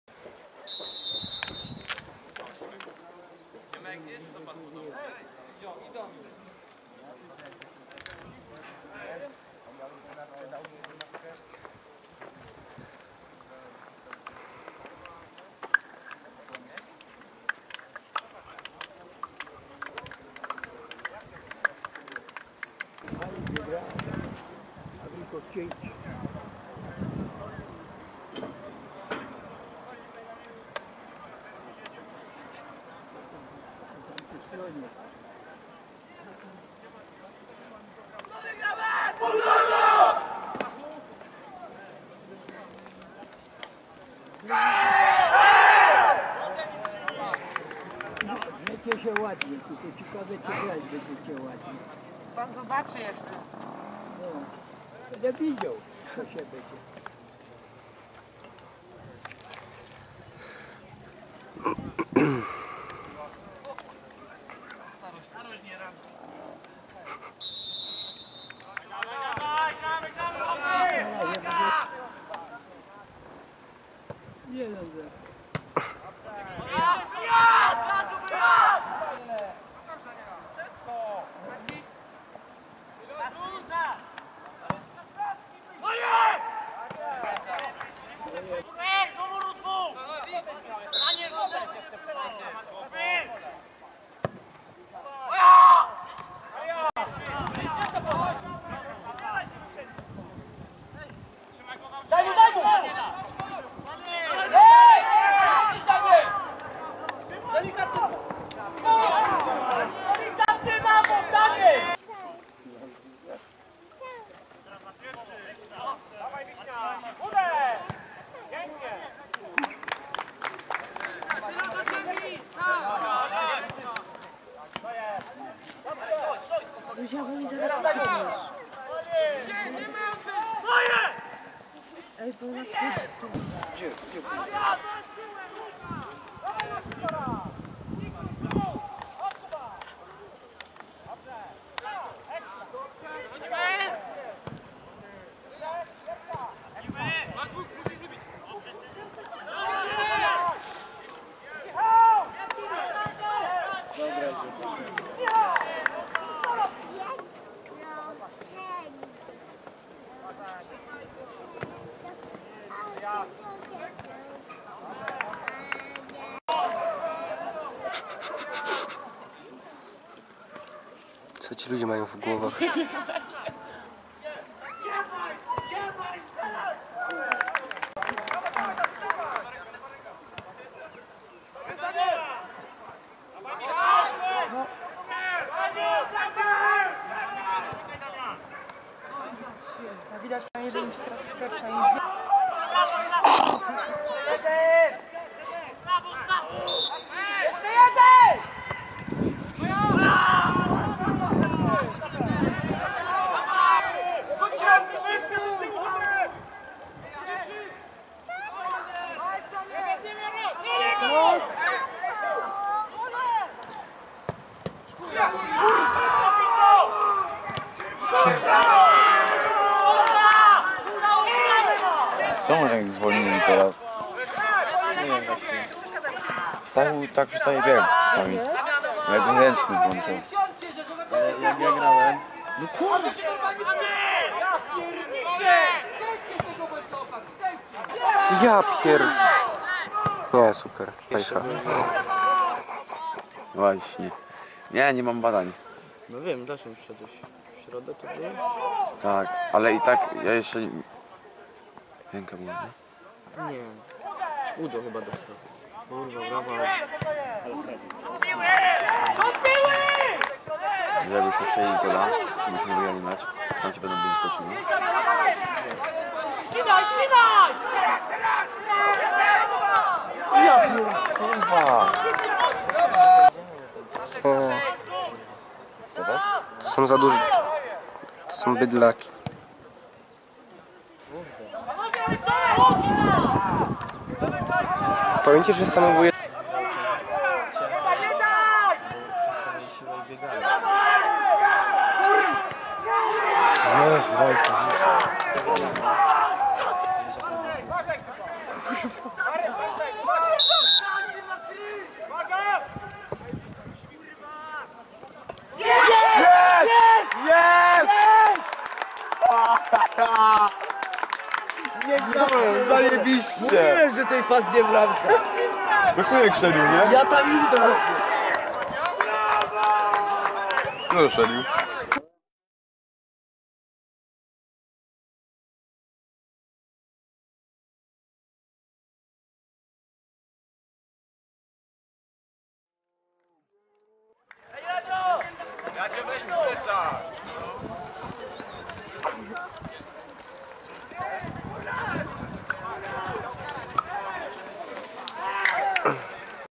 Bolesław Bukowno - KS Olkusz (Juniorzy Starsi) 02.04.2016
Mecz 1. kolejki grupy spadkowej I Ligi Okręgowej Juniorów Starszych podokręgu Kraków Bolesław Bukowno - KS Olkusz. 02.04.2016, Bukowno ul. Spacerowa 1. Zapraszam do oglądania skrótu.